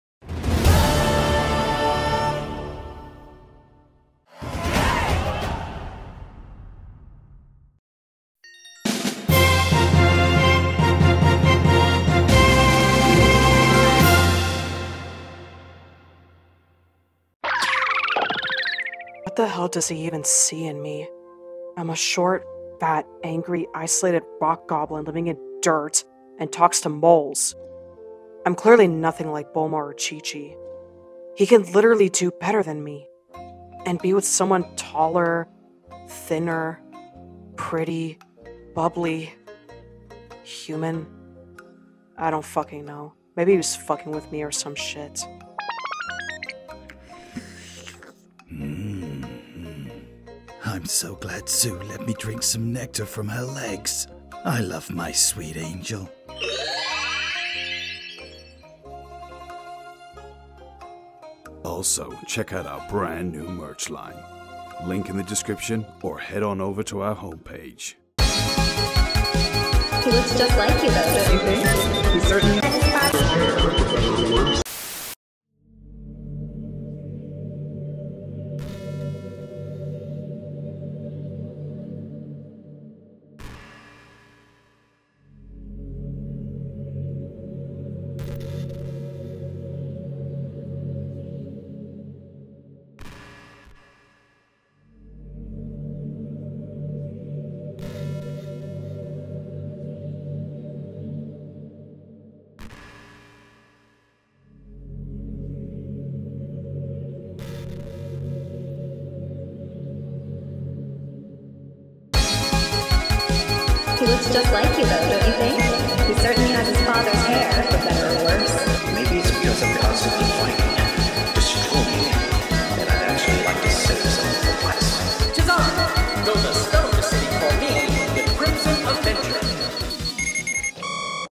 WHY Turles is Drawn to Her? Dragon Ball Comic Dub
🔞 For The Uncensored NSFW Version

💥 Tsu-Chi is Insecure About Turles' Feelings... But Why?! 💥

In this emotional Dragon Ball Comic Dub, Tsu-Chi struggles with deep insecurities as she questions why Turles, the powerful Saiyan warrior, is drawn to her. Is there something special about her, or is she overthinking? 🌸💭

👉 In this episode: We dive into Tsu-Chi’s inner thoughts and explore the real reason behind her doubts. Will she ever understand what Turles sees in her? It might even be something a little kinky. 🦶

Comic, Thumbnail Art and Voice of Tsu-Chi
🎨🎙️ : SketchPooch

Turles
🎙️ : Dandelion & Bardock